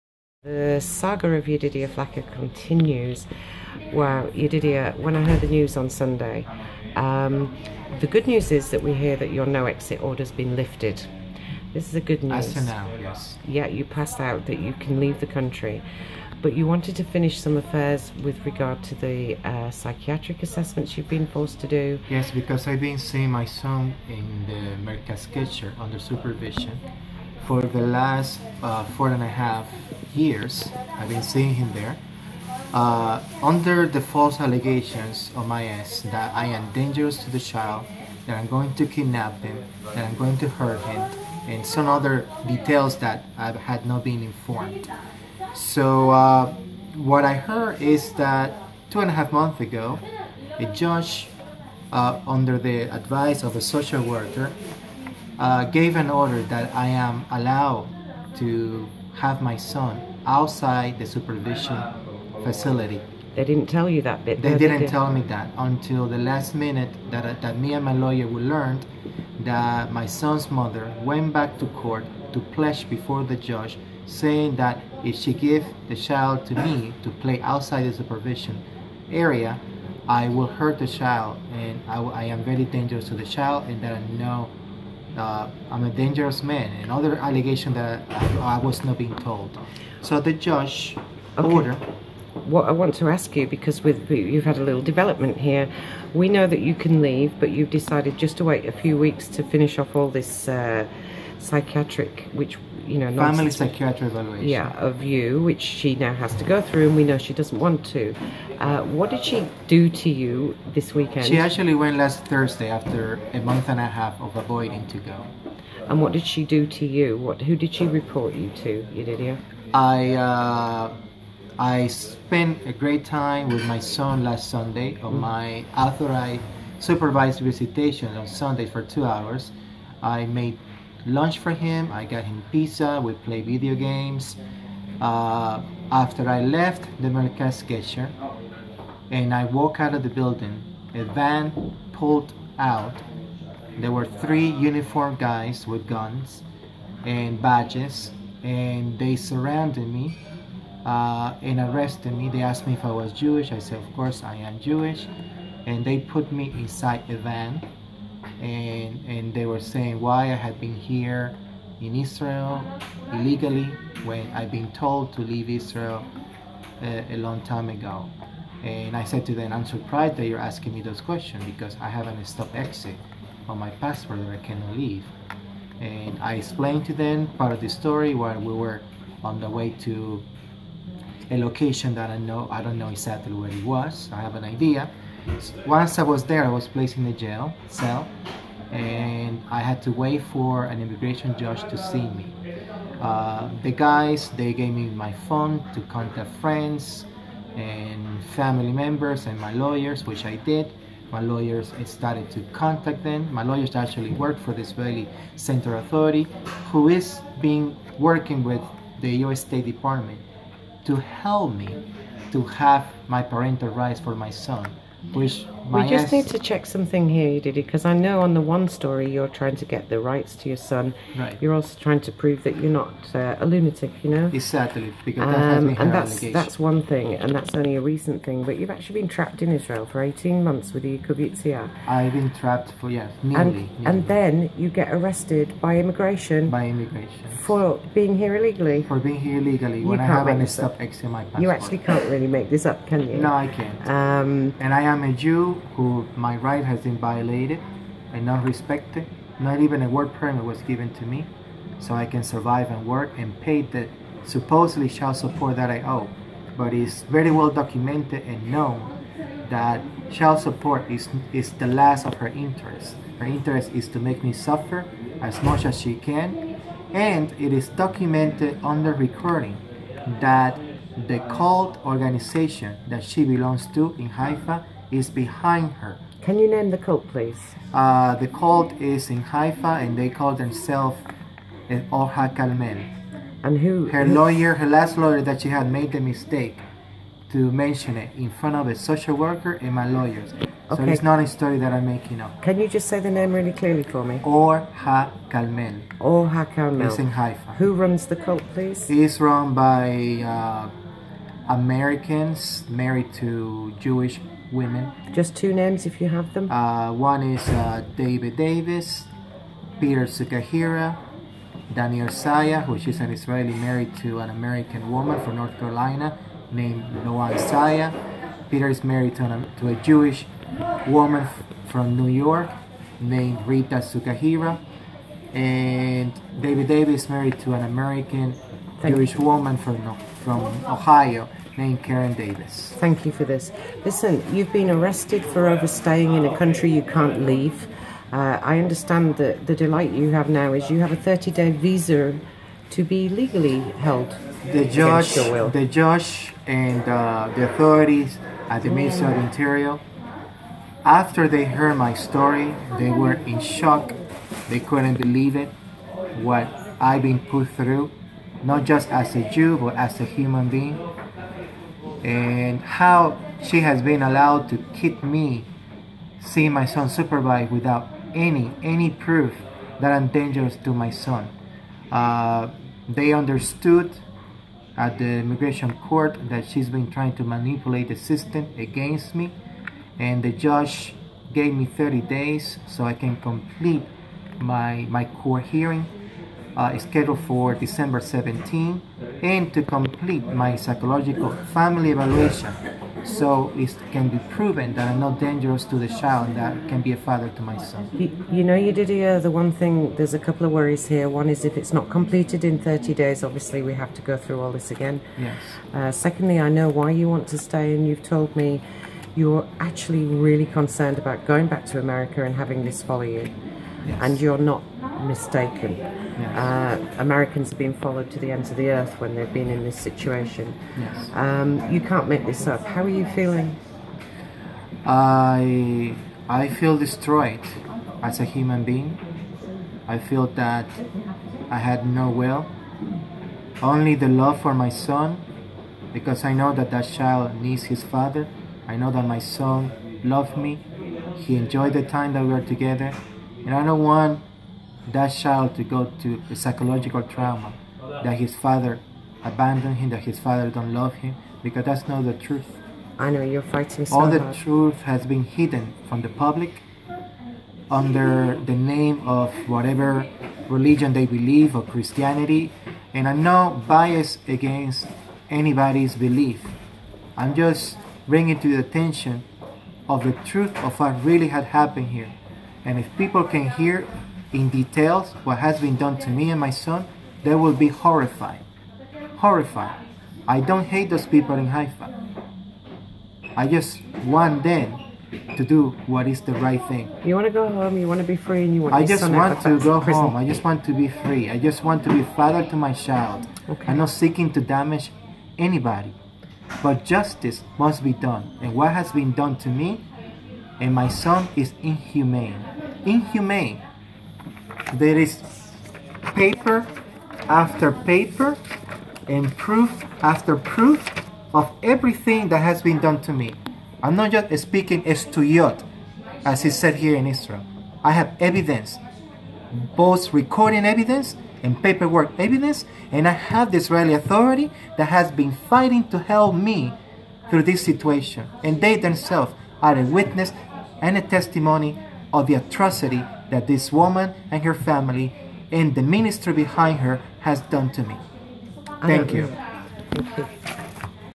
US CITIZEN TRAPPED IN ISRAEL SENT TO JAIL FOR NOT LEAVING!!
Yedidya Flaquer, trapped in Israel for 18 months, was arrested for overstaying without a visa!  He was prevented from leaving, held against his will, and then his ex wife reported him to Immigration.  How is this madness allowed for foreign nationals??